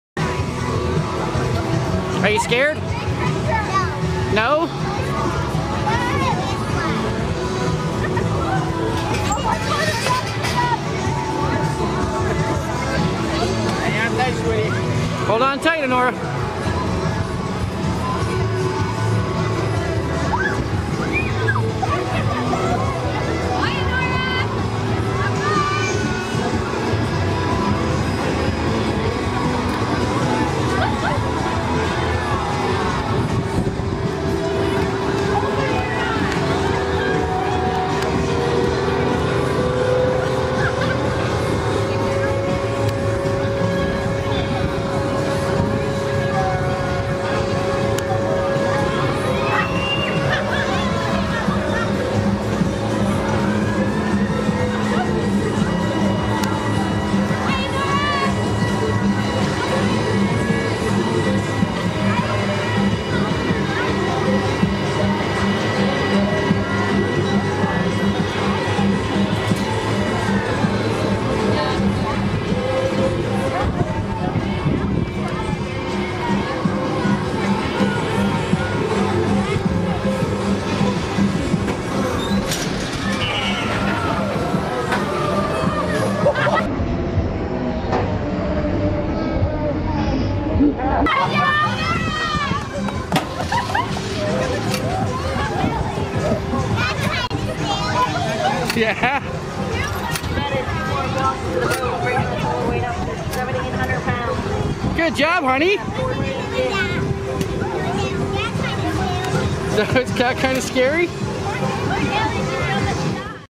5 year old goes on ride at Fair I would be too afraid to go on. Hang On Tight Sweetie
This is her reaction to being on a Carnival ride that I wouldn't be brave enough to go on caught on video. Back Story - She begged and begged us to go on this ride. Thinking there was no way she was tall enough yet.  I told her. "Only if you're tall enough" Little did I know She was going to be the just the perfect height. So how could I say no.